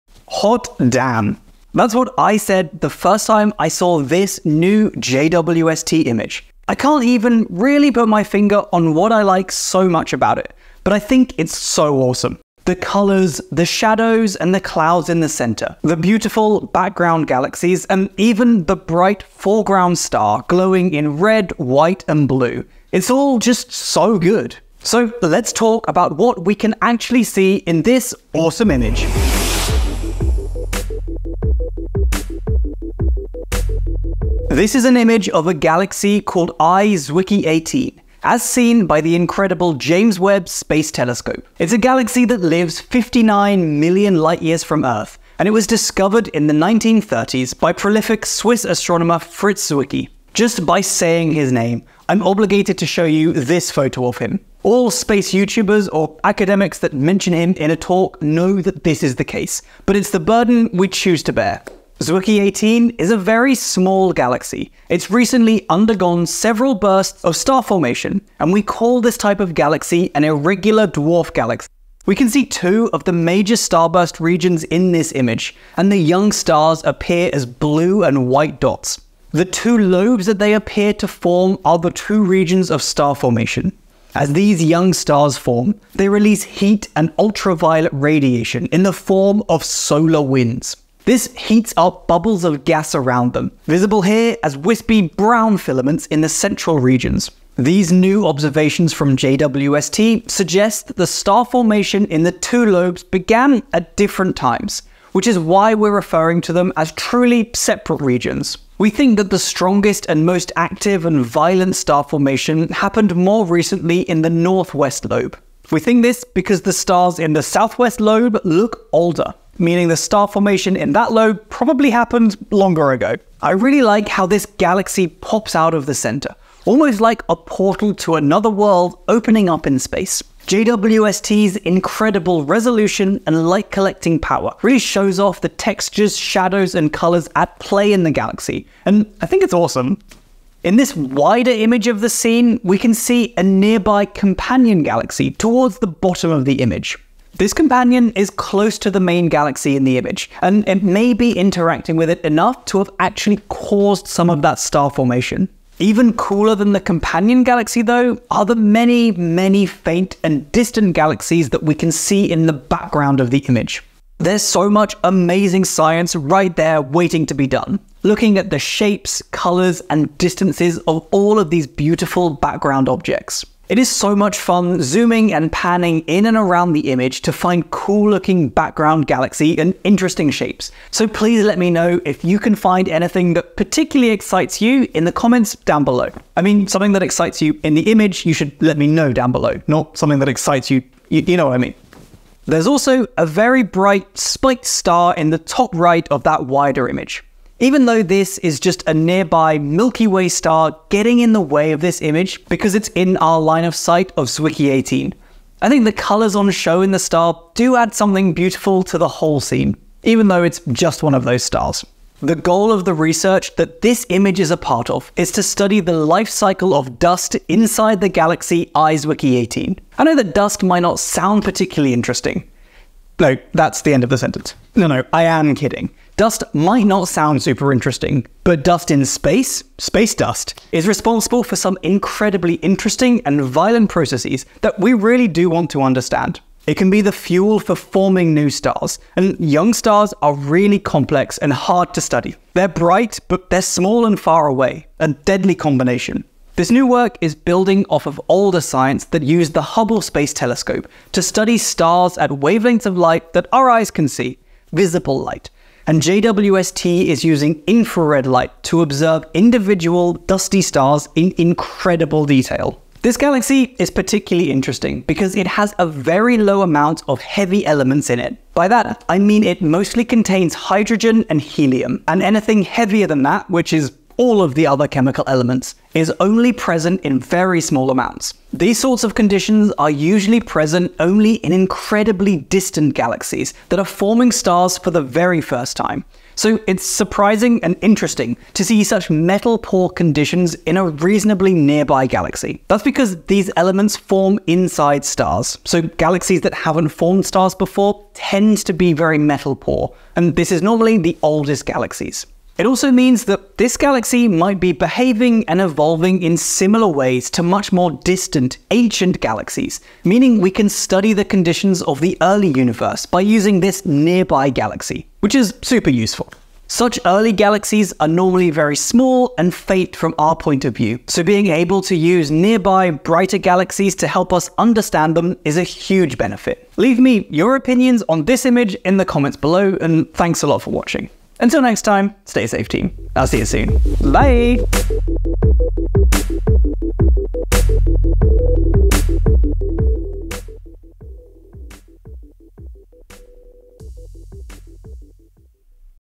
Incredible NEW JWST Galaxy Blew My Mind!
JWST just imaged this stunning nearby galaxy, called I Zwicky 18. It's an irrelgular dwarf galaxythat is absolutely beautiful, has a composition like galaxies in the very early universe, and (most importantly) looks like a portal in space! Let's talk about what we can see in this image, and why we took it!

Hey team!